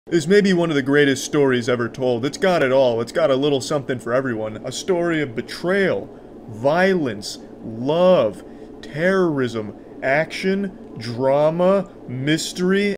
Cr1tiKal's thoughts on Tiny Taco Tales
Penquinz0 cr1tikal judges Tiny Taco Tales as a moist meter and gives his honest thoughts